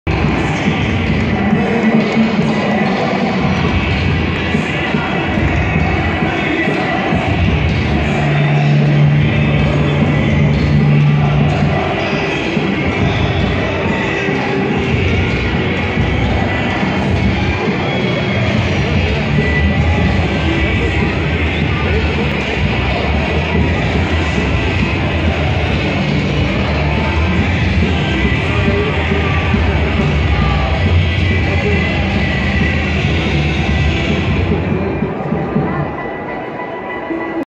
Ultra Tuza despide al Equipo después de su triunfo contra Mazatlán
En el arranque de la Jornada 7 del Clausura 2022. Pachuca venció por marcador de 3-1 al Mazatlán FC.

La Afición y la Ultra Tuza se han vuelto a conectar con el equipo que momentáneamente es el Líder General.